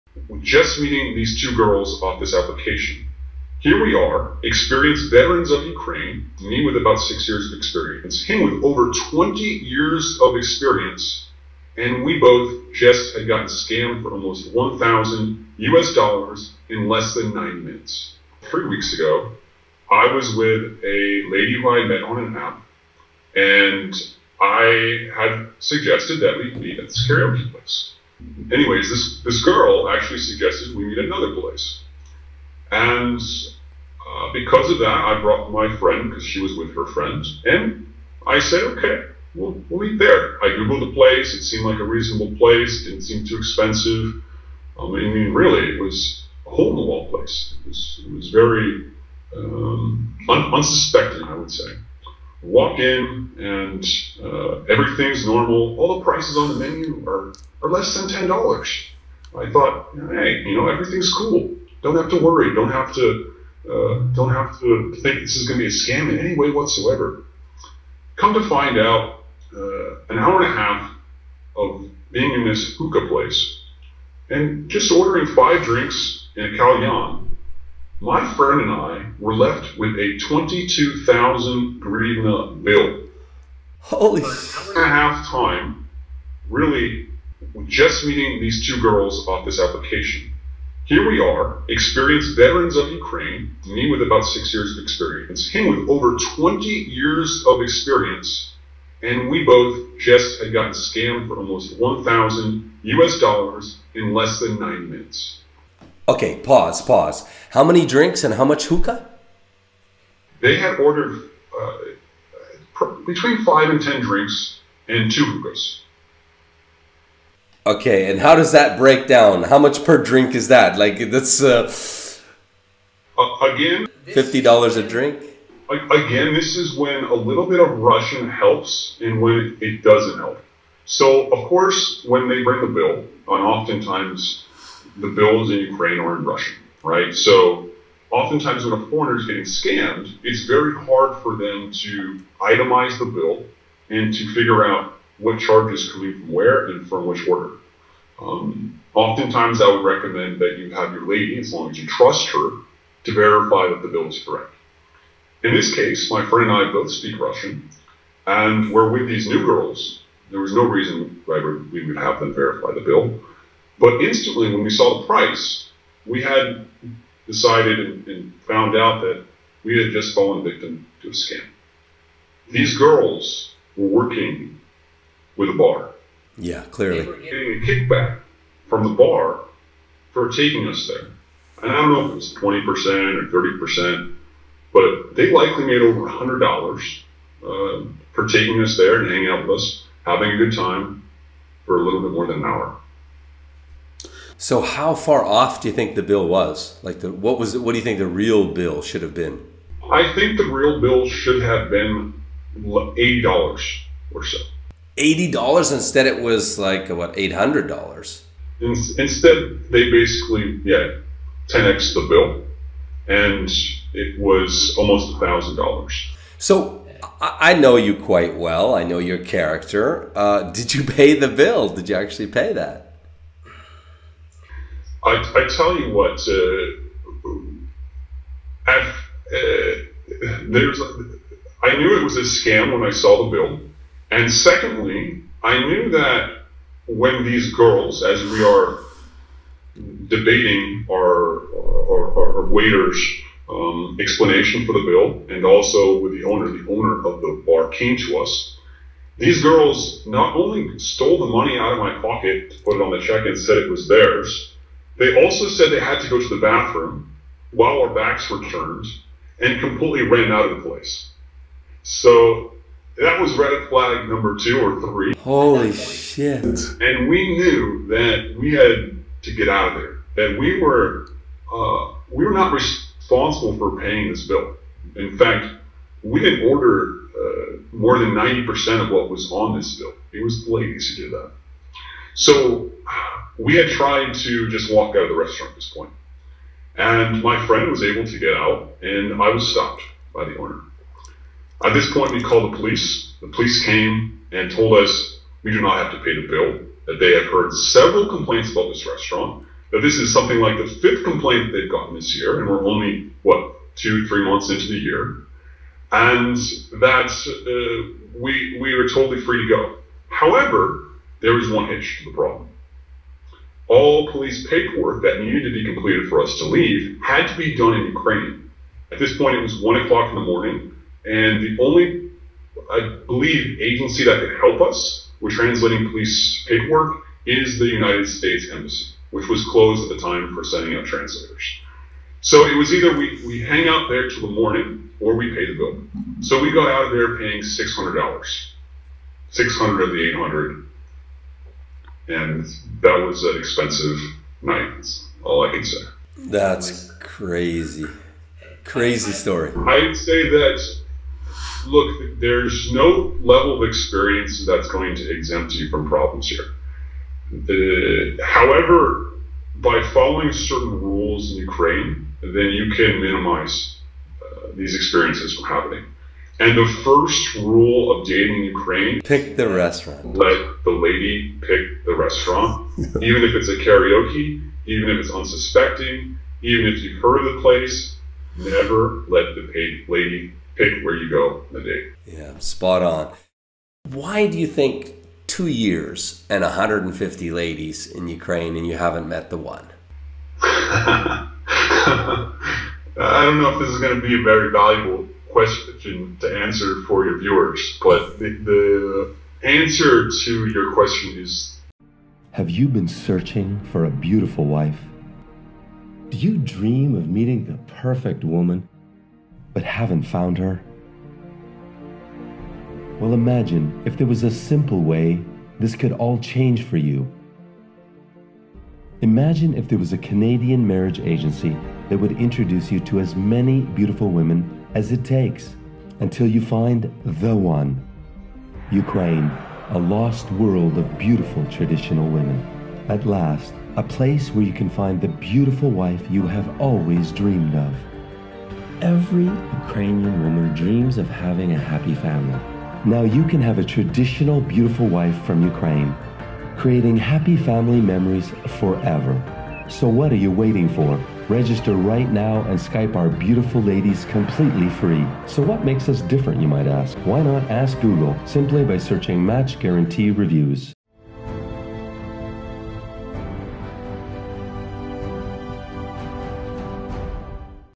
Do You Believe Dating In Ukraine Is Honest?
Mr. "X" has spent $50,000 dating in Ukraine meeting 150 Beautiful Ukrainian girls. In episode 1, he shares how he was recently scammed out of $1,000 by a Tinder date in 90 minutes.

3 weeks ago, I was with a lady, who I met on Tinder app and I had suggested that we'd be at this karaoke place. Anyways, this girl actually suggested we need another place and because of that, I brought my friend. Because she was with her friends and I said okay we live there. I googled the place it seemed like a reasonable place, didn't seem too expensive. I mean, really it was a hole in the wall place it was very unsuspecting, I would say.

We walk in and everything is normal, all the prices on the menu are less than ten dollars. I thought, hey, you know everything's cool, I don't have to worry, don't have to think this is gonna be a scam. Whatsoever come to find out in an hour and a half of being in this hookah place and just ordering five drinks in a calyan, my friend and I were left with a 22,000 grivnas bill. Holy second and a half time really just meeting these two girls off this application here.

We are experienced "veterans" of Ukraine. Me with about six years of experience, my friend with over 20 years of experience and we both just had gotten scammed for almost $1,000 in less than nine minutes!

How many drinks and how much hookah they had ordered?

Between five and ten drinks and two hookahs.

 Okay, and how does that break down? How much per drink? Was it about fifty dollars a drink?

Again, this is when a little bit of Russian helps and when it doesn't help. So, of course, when they break a bill on... oftentimes the bills in Ukraine are in Russian, right so oftentimes when the foreigners getting scammed it's very hard for them to itemize the bill and to figure out what charges coming from. I would recommend that you have your lady as long as you trust her, to verify that the bill is correct. In this case my friend and I both speak Russian and we're with these new girls there was no reason, why we would have them verify the bill?

But instantly when we saw the price we had decided and found out that we had just fallen victim to a scam. These girls were working with a bar.